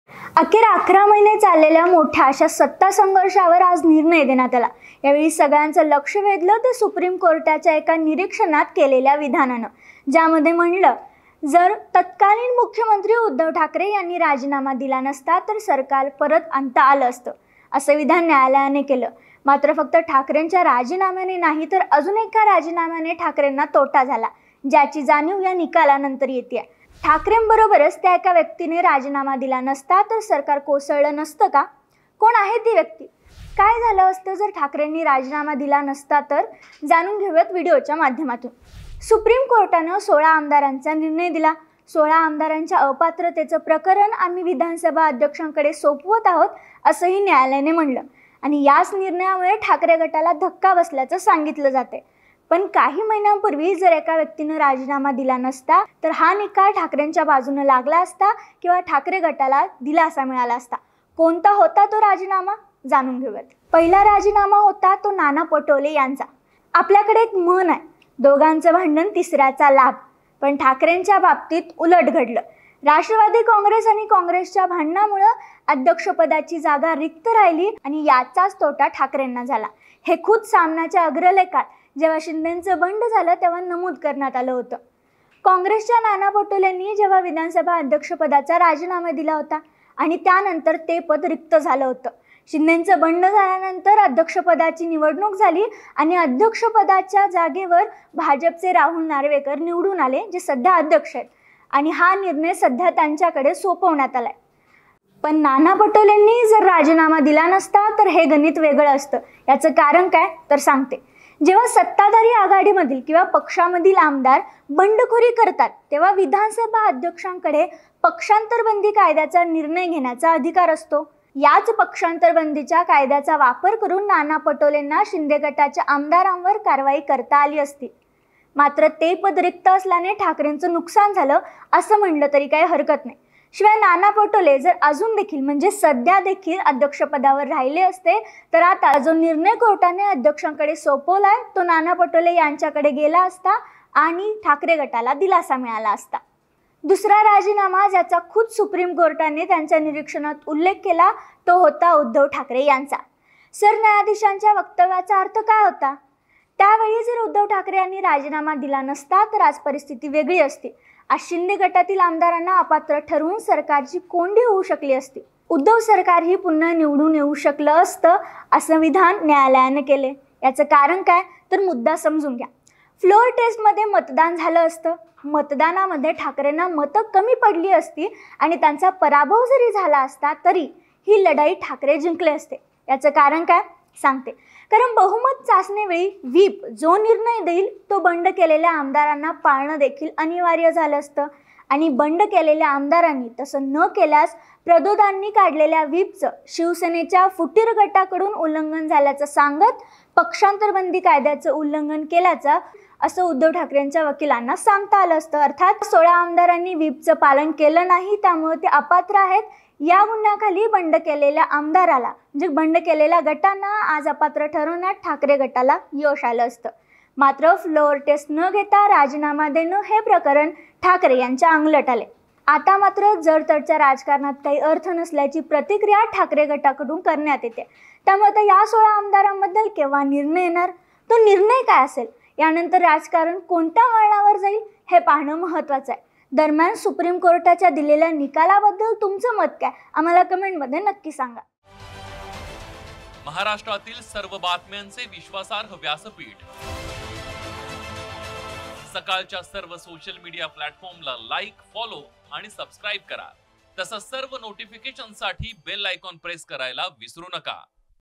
Supreme Court Decision | Uddhav Thackeray आणि Nana Patole यांनी राजीनामा देऊन चूक केली का?
न्यायालयाने केलेल्या विधानानं सगळ्याचं लक्ष वेधलं. ज्यामध्ये सुप्रीम कोर्ट म्हणालं की जर तत्कालीन मुख्यमंत्री उद्धव ठाकरे यांनी राजीनामा दिला नसता, तर सरकार परत आणलं असतं, असं विधान केलं होतं मात्र फक्त ठाकरेंच्या राजीनाम्यानं नाही तर अजून एका राजीनाम्यानं ठाकरेंना तोटा झाला ज्याची जाणीव या निकालानतर येत आहे.ठाकरेंसोबत त्या एका व्यक्तीनं राजीनामा दिला नसता तर सरकरा कोसळलं नसत का? कोण आहे ती व्यक्ती ?काय झालं असंत जर ठाकरेंनी राजीनाम दिला नसता तर? जाणून घेऊयाच व्हिडिओच्या माध्यमातून
SH, YL_0523